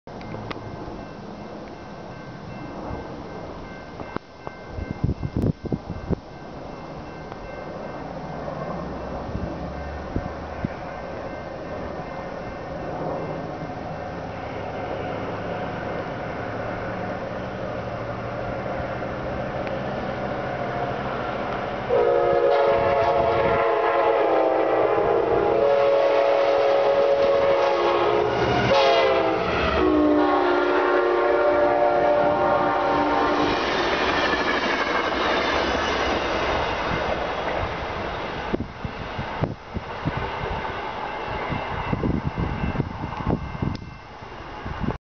A very late Sounder #1503 by Sumner
Video of Sounder #1503, running nearly an hour late due to frozen switches and down trees along the Seattle Subdivision. Supposedly this train was also heavily delayed due to dispatching issues and not releasing the train until Amtrak #513 came North first.